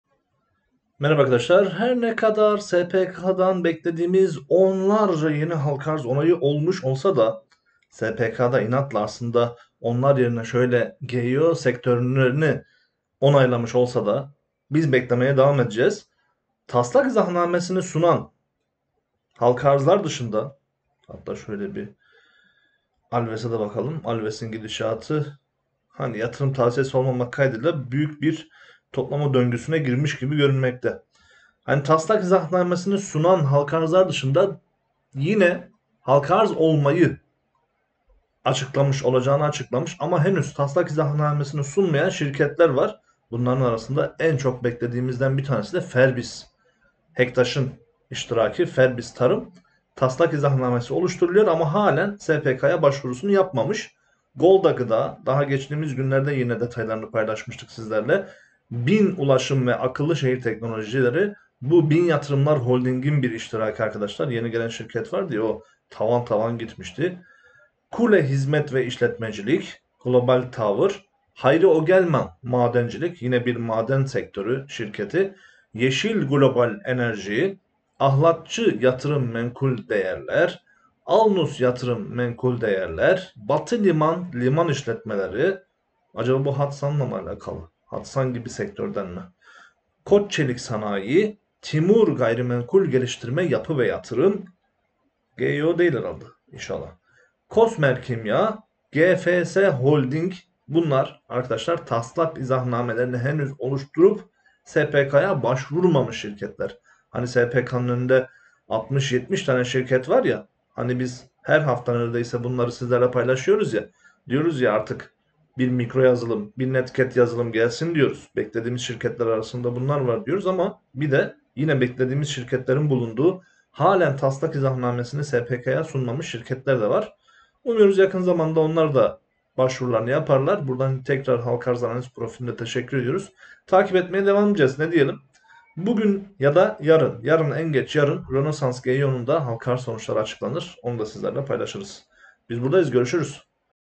SPK Başvurusu Yapmamış Halka Arz Olacak Şirketler
Hani her hafta paylaşıyoruz ya bu hafta halka arz onayı beklediğimiz şirketler diye bir de daha SPK başvuru yapmamış ama halka arz olacağını açıklamış şirketler de var.